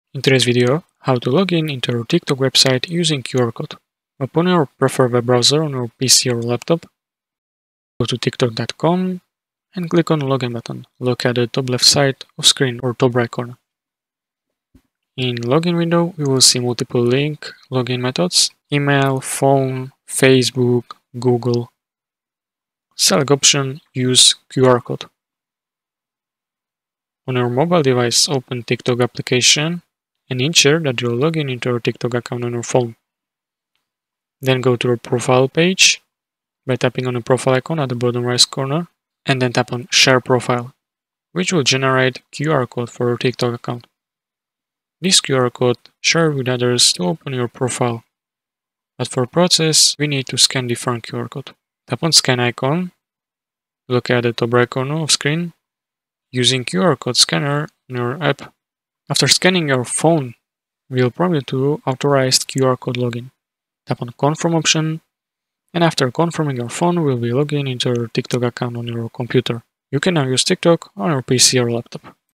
How To Log into TikTok Website Using a QR Code (2024)
Thank you very much for your support!

► SAMSUNG Odyssey G50A

How to Log into TikTok Website Using a QR Code

Logging into TikTok on your PC or laptop using a QR code is a seamless and secure method. Here is a step-by-step guide to help you through the process:

Step 1: Open TikTok Website on Your PC or Laptop

1. **Launch Web Browser**:
   - Open your preferred web browser on your PC or laptop.

3. **Access Login Options**:
   - Click on the "Login" button located at the left side of the screen or the top right corner.

Step 2: Select QR Code Login

1. **Choose QR Code Option**:
   - In the login window, you will see multiple login methods (email, phone, Facebook, Google, etc.).
   - Select the option "Use QR code."

Step 3: Open TikTok App on Your Phone

1. **Open TikTok App**:
   - On your mobile device, open the TikTok application.
   - Ensure you are logged into your TikTok account on your phone.

2. **Navigate to Your Profile**:
   - Go to your profile by tapping the "Profile" icon at the bottom right of the screen.

3. **Generate QR Code**:
   - Tap on "Share profile" which will generate a QR code for your TikTok account.
   - Note: This QR code can be shared with others to open your profile, but for this process, we need to scan a different QR code.

4. **Open Scanner**:
   - Tap on the scan icon located at the top right corner of the screen (it looks like a small QR code scanner).

Step 4: Scan QR Code on Computer

1. **Scan the QR Code**:
   - Using the QR code scanner on your TikTok app, scan the QR code displayed on your computer screen.

2. **Authorize Login**:
   - After scanning, your phone will prompt you to authorize the QR code login.
   - Tap on the "Confirm" option.

Step 5: Complete Login

1. **Login Confirmation**:
   - After confirming on your phone, you will be logged into your TikTok account on your computer.
   - You can now use TikTok on your PC or laptop.

Summary

By following these steps, you can easily log into the TikTok website using a QR code generated from the TikTok app on your phone. This method is quick, secure, and convenient, allowing you to access your TikTok account on your computer without needing to type in your credentials.